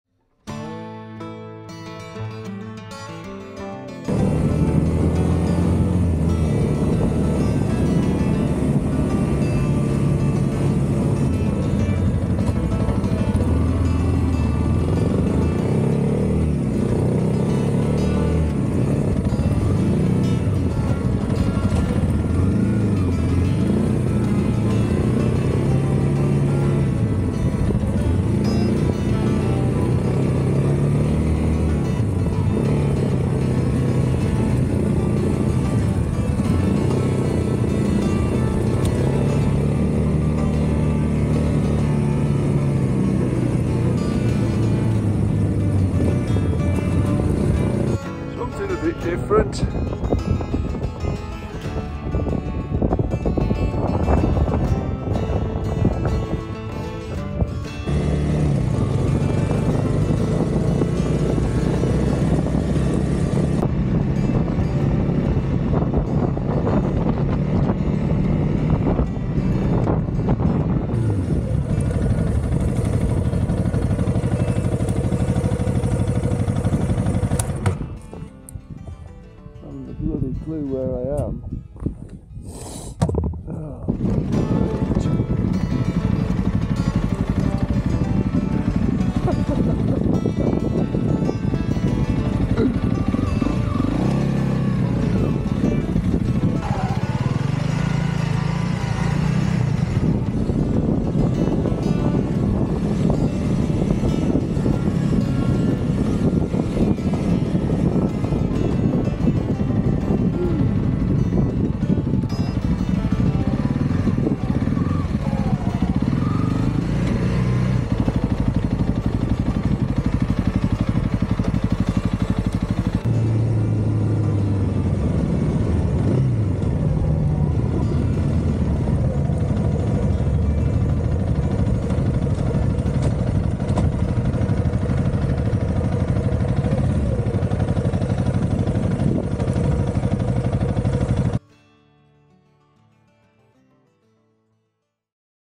A novice off road on a Honda CRF300 - making the most of the days before chemo 4/6
My chemotherapy is every three weeks, the first week after is a bit bumpy but things improve during weeks two and three. Being diagnosed with inoperable cancer makes me want to do what I can while I can, hence a dabble with some minor off road biking.